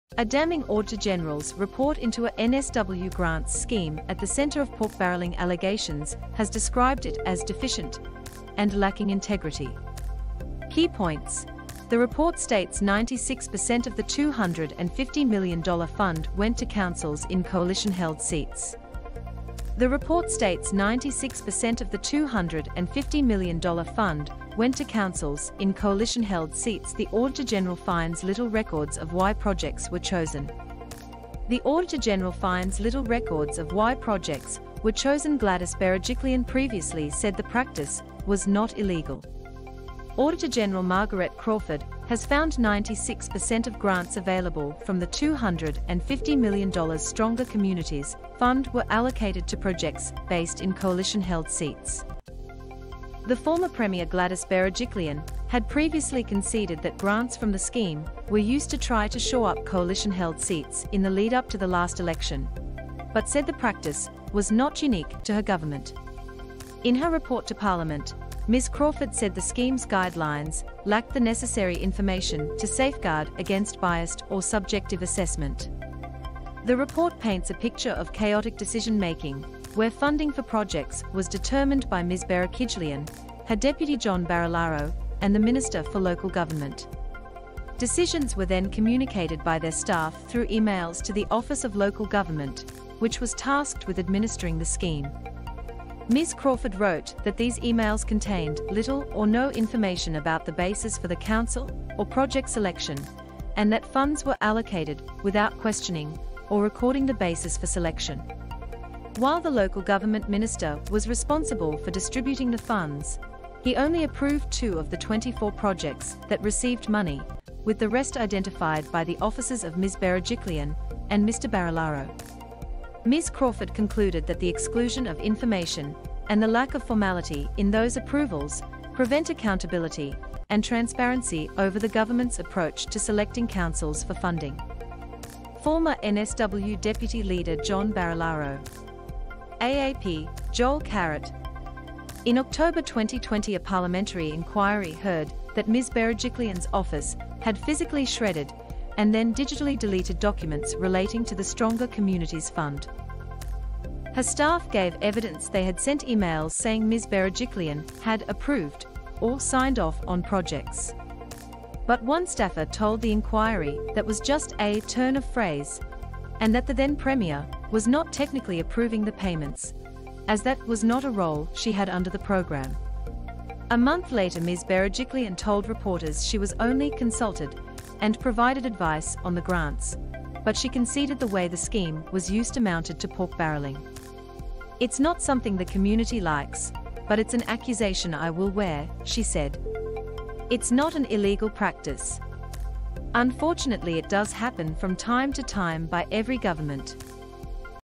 NSW auditor general releases damning report on government grant scheme at pork barrel claims centre
A report by the NSW auditor-general into a $250 million NSW grants scheme paints a picture of chaotic decision-making, where funding for projects was determined by former premier Gladys Berekijlian and her deputy John Barilaro.

A damning auditor-general's report into a NSW grants scheme at the centre of pork-barrelling allegations has described it as "deficient" and "lacking integrity".

Key points: The report states 96 per cent of the $250m fund went to councils in Coalition-held seats

The report states 96 per cent of the $250m fund went to councils in Coalition-held seats The auditor-general finds little records of why projects were chosen

The auditor-general finds little records of why projects were chosen Gladys Berejiklian previously said the practice was "not illegal"

Auditor-general Margaret Crawford has found 96 per cent of grants available from the $250 million Stronger Communities Fund were allocated to projects based in Coalition-held seats.

The former premier Gladys Berejiklian had previously conceded that grants from the scheme were used to try to shore up Coalition-held seats in the lead up to the last election, but said the practice was "not unique" to her government.

In her report to parliament, Ms Crawford said the scheme's guidelines "lacked the necessary information to safeguard against biased or subjective assessment".

The report paints a picture of chaotic decision making, where funding for projects was determined by Ms Berekijlian, her deputy John Barilaro and the minister for local government.

Decisions were then communicated by their staff through emails to the Office of Local Government, which was tasked with administering the scheme.

Ms Crawford wrote that these emails contained "little or no information about the basis for the council or project selection" and that funds were allocated "without questioning or recording the basis for selection".

While the local government minister was responsible for distributing the funds, he only approved two of the 24 projects that received money — with the rest identified by the offices of Ms Berejiklian and Mr Barilaro.

Ms Crawford concluded that the exclusion of information and the lack of formality in those approvals "prevent accountability and transparency over the government's approach to selecting councils for funding".

Former NSW deputy leader John Barilaro. ( AAP: Joel Carrett )

In October 2020 a parliamentary inquiry heard that Ms Berejiklian's office had physically shredded and then digitally deleted documents relating to the Stronger Communities Fund.

Her staff gave evidence they had sent emails saying Ms Berejiklian had "approved" or "signed off" on projects.

But one staffer told the inquiry that was just a "turn of phrase" and that the then premier was not technically approving the payments as that "was not a role she had under the program".

A month later Ms Berejiklian told reporters she was only "consulted" and "provided advice" on the grants, but she conceded the way the scheme was used amounted to pork barrelling.

"It's not something the community likes ... but it's an accusation I will wear," she said.

"It's not an illegal practice. Unfortunately it does happen from time to time by every government."